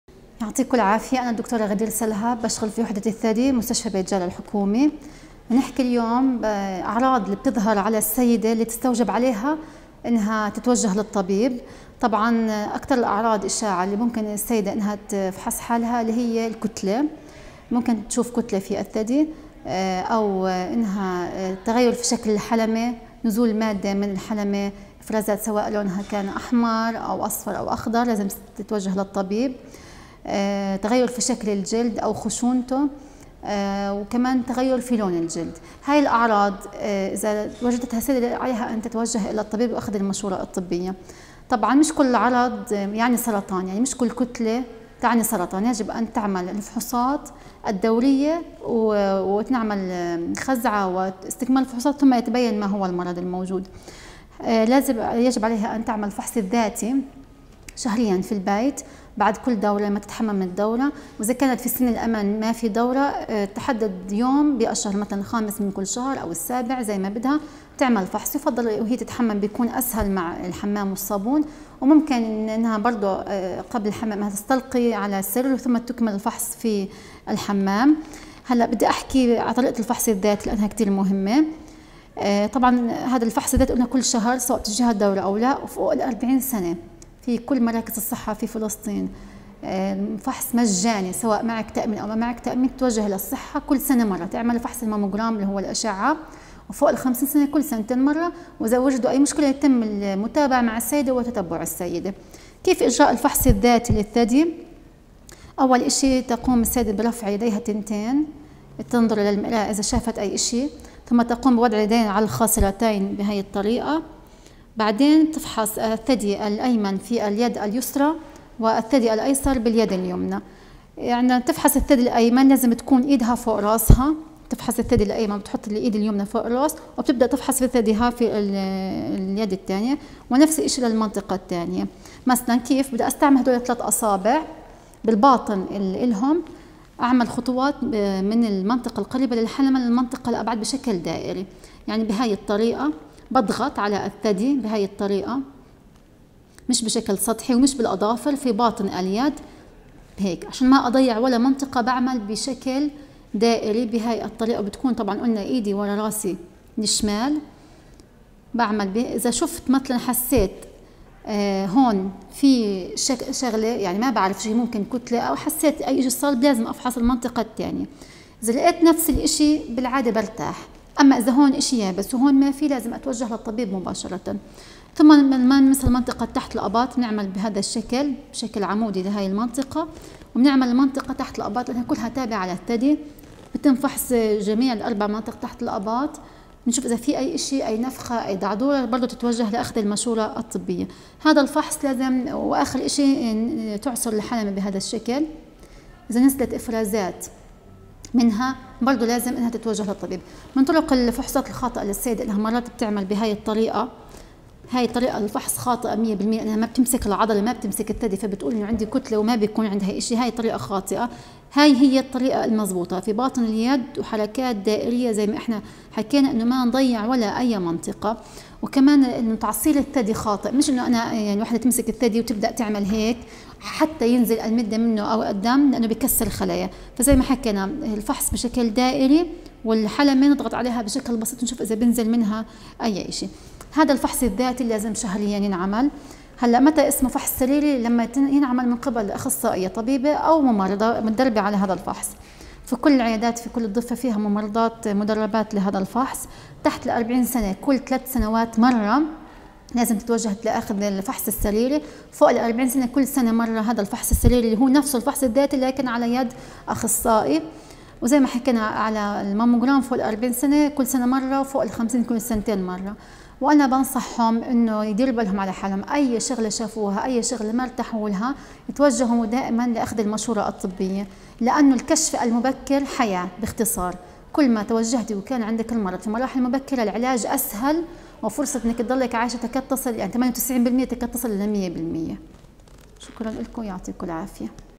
تعرفي على اعراض سرطان الثدي واهمية الفحص المبكر
بتنظيم النادي الصحي في مركز لاجىء وبالتعاون مع الدكتورة غدير سلهب